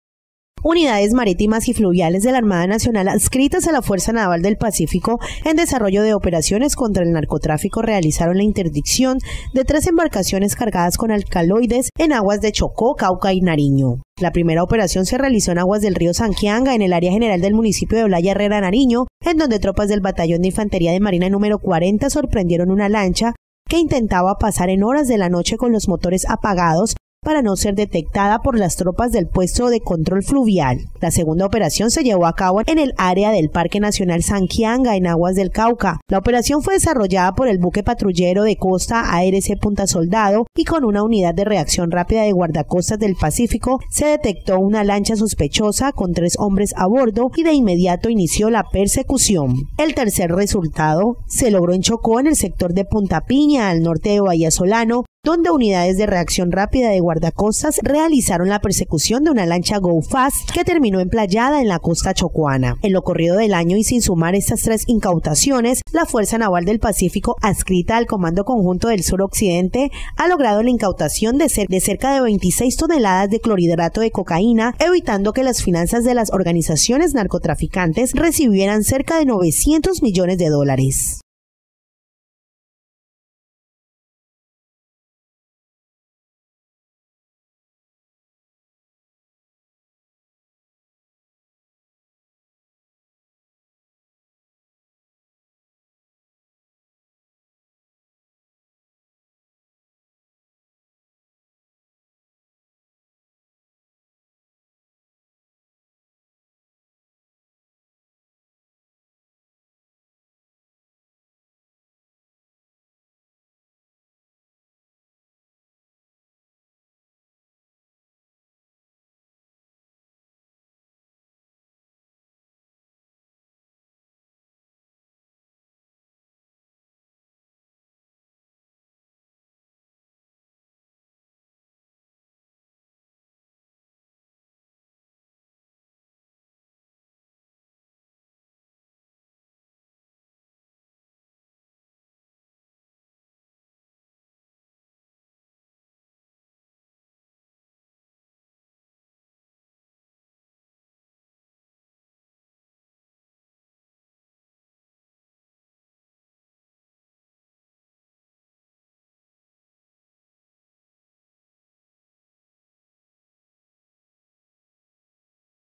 ARMADA INCAUTA 26 TONELADAS DE COCAINA E INCAUTA TRES CARGAMENTOS DE ALCALOIDES EN EL PACÍFICO.
en desarrollo de operaciones contra el narcotráfico realizaron la interdicción de tres embarcaciones cargadas con alcaloides, en aguas de Chocó, Cauca y Nariño.   La primera operación se realizó en aguas del río Sanquianga, en área general del municipio de Olaya Herrera – Nariño, en donde tropas del Batallón de Infantería de Marina No.40 sorprendieron una lancha que intentaba pasar en horas de la noche con los motores apagados para no ser detectada por la tropas del puesto de control fluvial.La segunda operación se llevó a cabo en área del Parque Nacional Sanquianga – en aguas del Cauca; la operación, fue desarrollada por el buque patrullero de costa ARC “Punta Soldado” y una Unidad de Reacción Rápida de Guardacostas del Pacífico, fue detectó una lancha sospechosa CON TRES HOMBRES A BORDO  y de inmediato inició la persecución.
 El tercer resultado se logró en Chocó en el sector de Punta Piña, al norte de Bahía donde Unidades de Reacción Rápida de Guardacostas realizaron la persecución de una lancha ‘Go Fast’ que terminó emplayada en la costa chocoana.
En lo corrido del año y sin sumar estas tres incautaciones, la Fuerza Naval del Pacífico adscrita al Comando Conjunto de Sur Occidente ha logrado la incautación de cerca de 26 toneladas de clorhidrato de cocaína, evitando que las finanzas de las organizaciones narcotraficantes recibieran cerca de 900 millones de dólares.